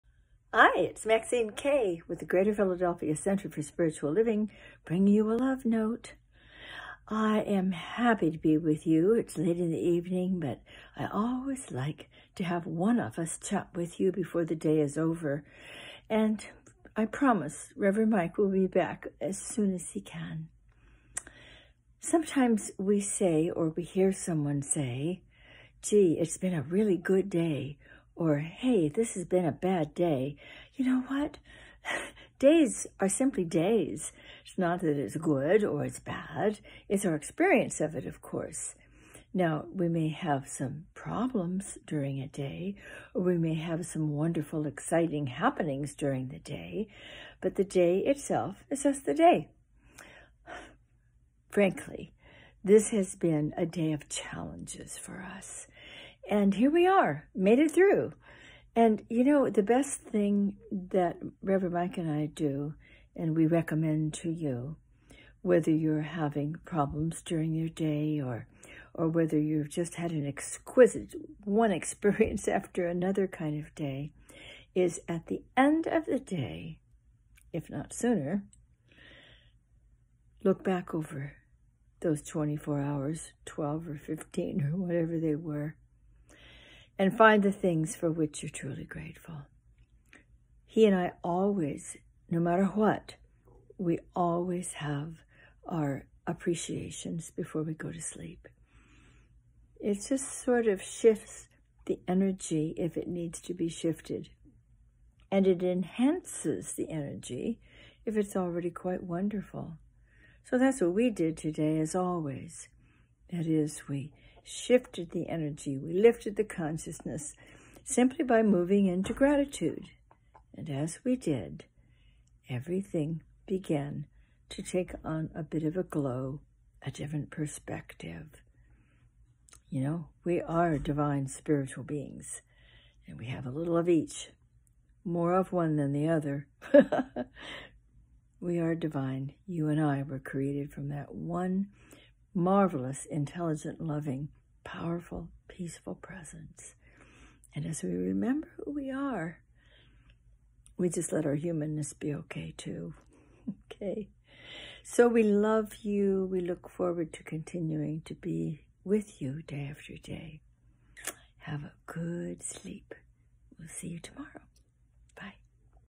Good Days, Bad Days?
It isn’t today but the perspective that makes it seem good or bad.￼

Even the energy of a challenging day may be lifted and transformed through renewing it with gratitude.